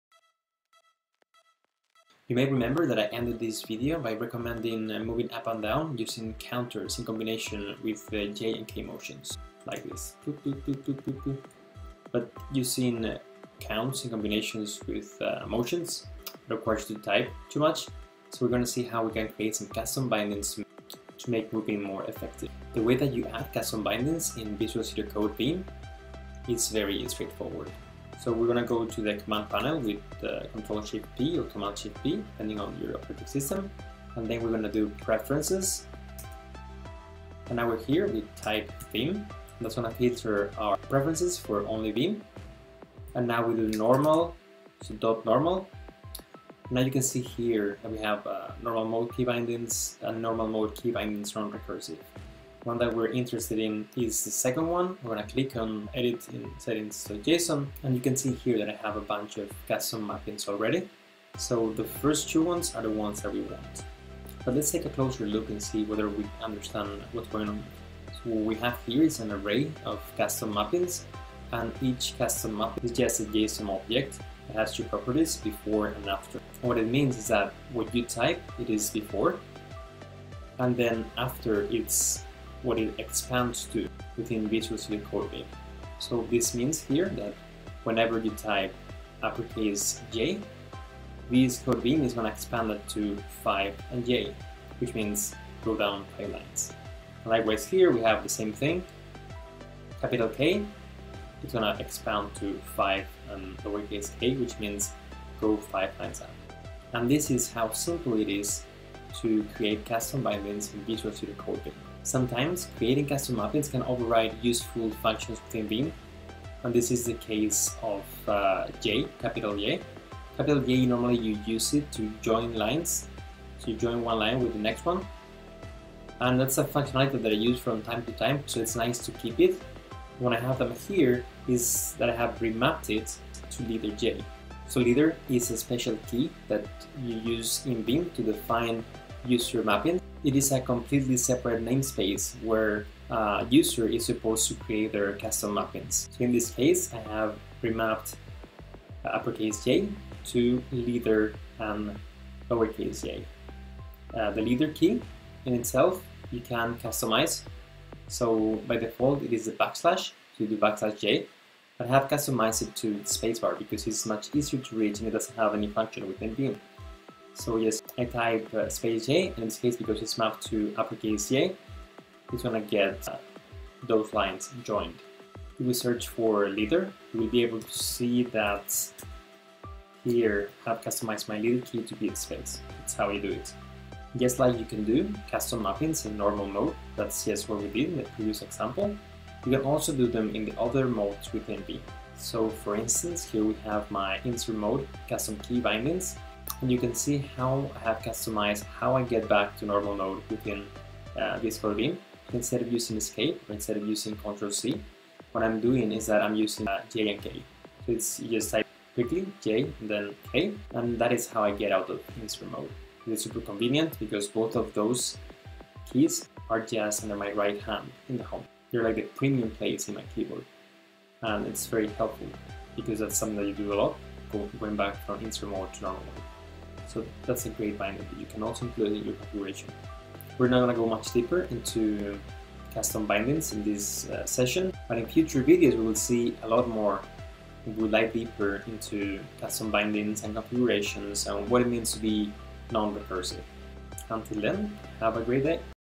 Custom mappings
Learn how to create custom mappings in VSCodeVim. Custom mappings are custom key combinations that map to Vim (or VSCode) features.

In this video you'll learn how to:

* Map J to 5j and K to 5k
* Keep the functionality of J (joining lines) near by remapping it to (Leader)j
* What is the (Leader) key and how to customize it
* Map jk to ESC so that you can exit insert mode swift like the wind

In general, create custom mappings when you want to bring some useful or often used feature within Vim to your finger tips. Prefer using the leader key for custom mappings but you can override some of Vim default bindings if you don't use them often.